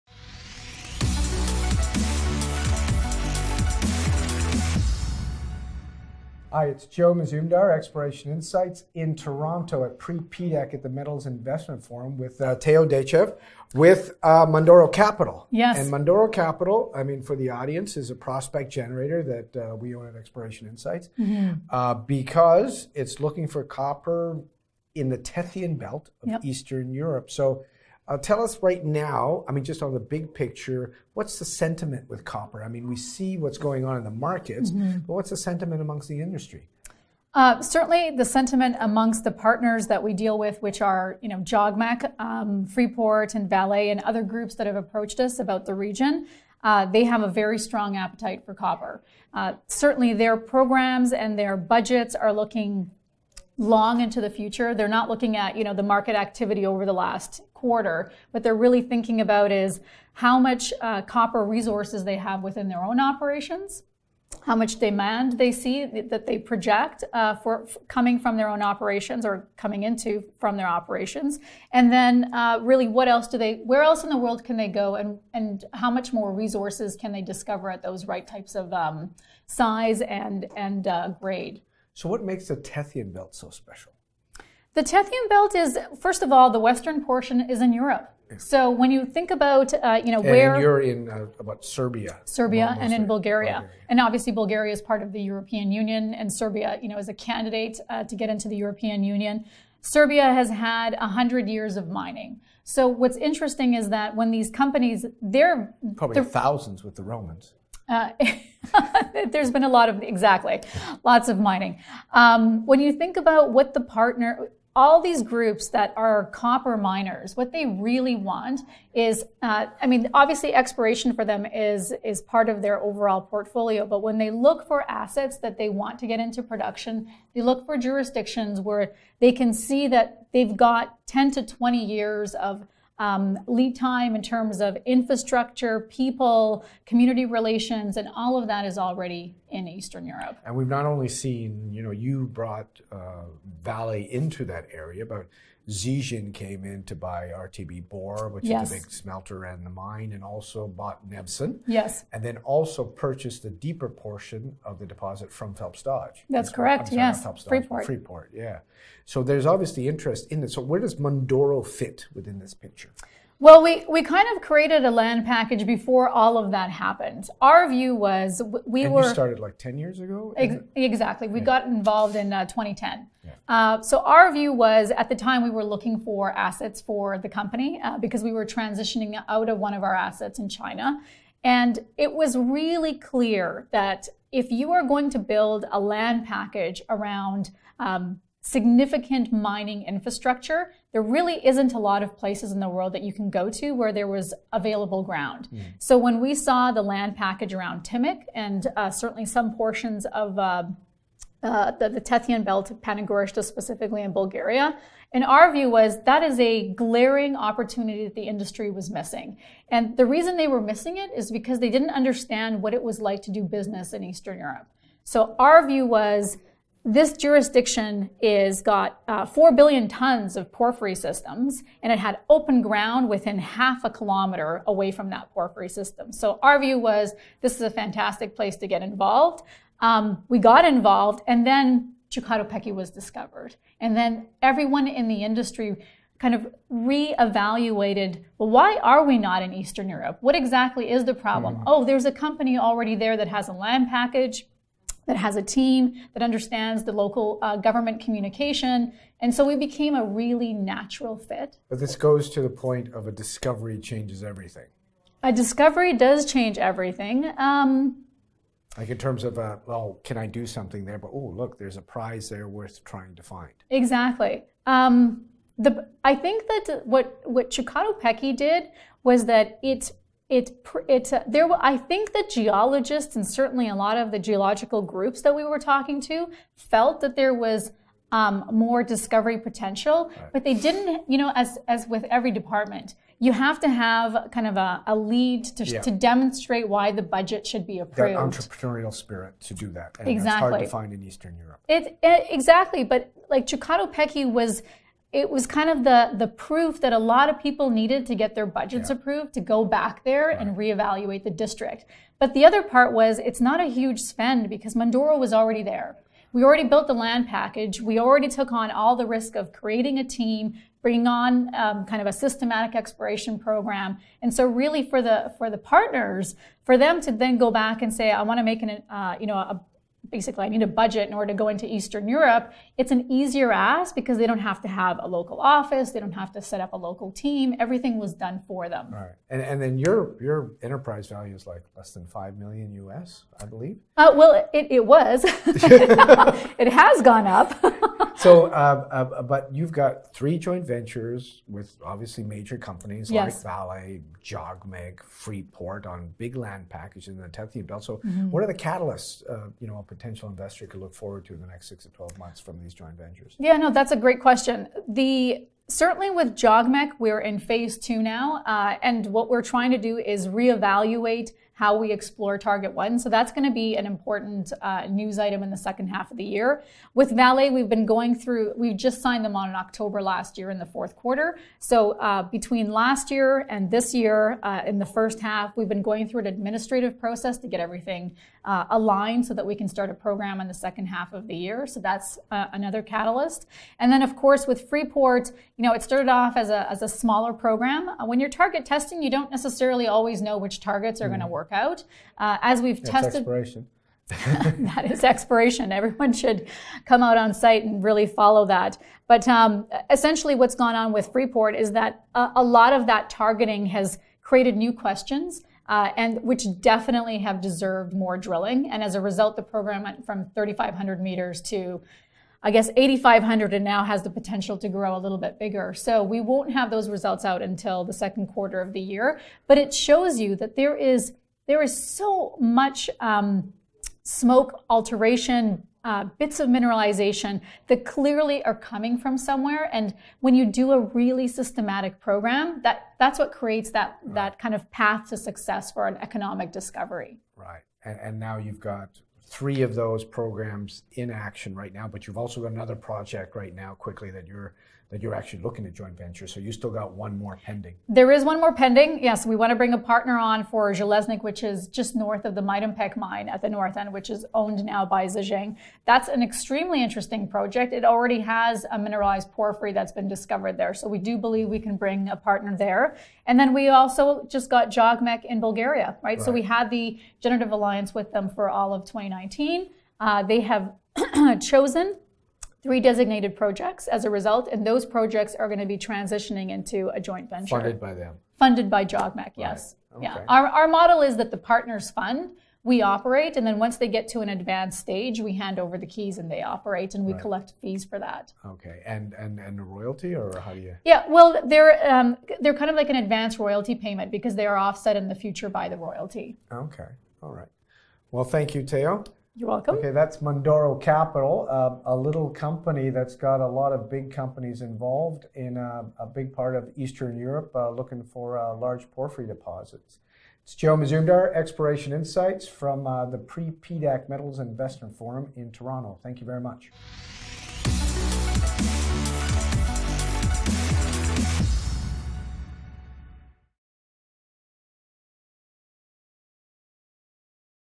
Joe Mazumdar talks to Teo Dechev of Mundoro Capital at Metals Investor Forum in Toronto.
Teo Dechev, CEO of Mundoro Capital (TSX-V: MUN) and Joe Mazumdar of Editor, Exploration Insights to discuss the general sentiment of copper as a whole and also the company's producing gold mine in Bulgaria and a feasibility stage gold project in China at Metals Investor Forum on February 28 - March 1, 2020 in Toronto. __________________________________________________________________________________

Don’t forget to sign up for Metals Investor Forum’s Video Library